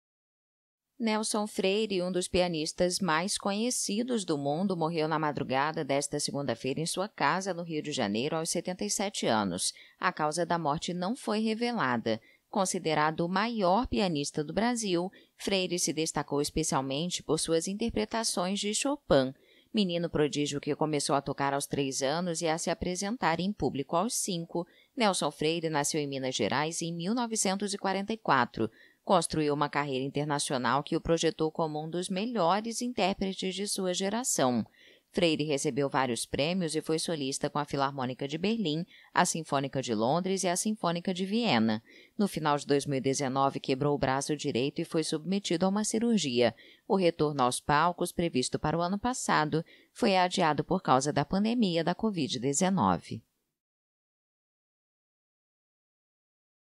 Morre Nelson Freire, célebre pianista brasileiro | AFP
O músico Nelson Freire morreu aos 77 anos no Rio de Janeiro. O pianista era um dos mais conhecidos do mundo e considerado um dos melhores intérpretes de sua geração.